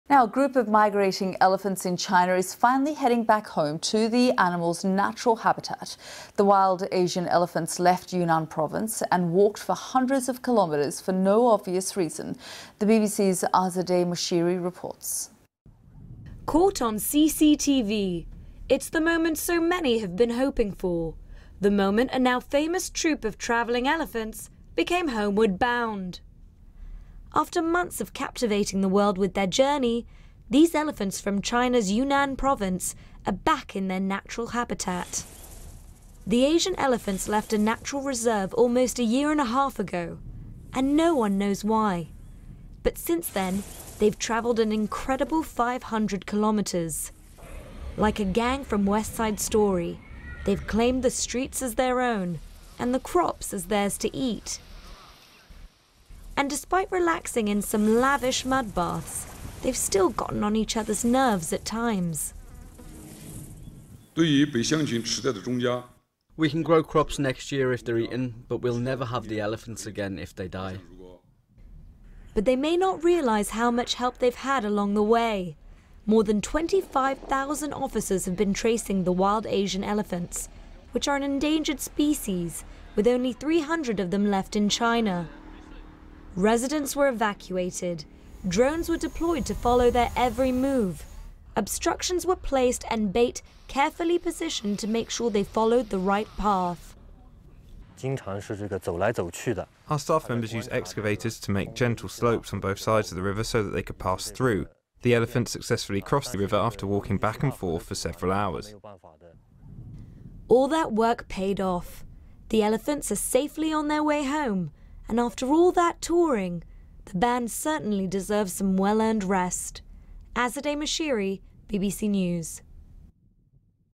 150,000 evacuated from path of trekking herd - BBC News
A herd of 14 elephants, who have made international headlines with their huge 500km (300-mile) trek across China, are finally on their way home.

State media say more than 25,000 police officers using vehicles and drones have monitored the herd, after the protected animals left a nature reserve in Yunnan about 17 months ago.

They've been through fields, towns and cities, eating millions of dollars worth of crops and damaging buildings, over the course of their journey and now, more than 150,000 people are being evacuated to make way for them.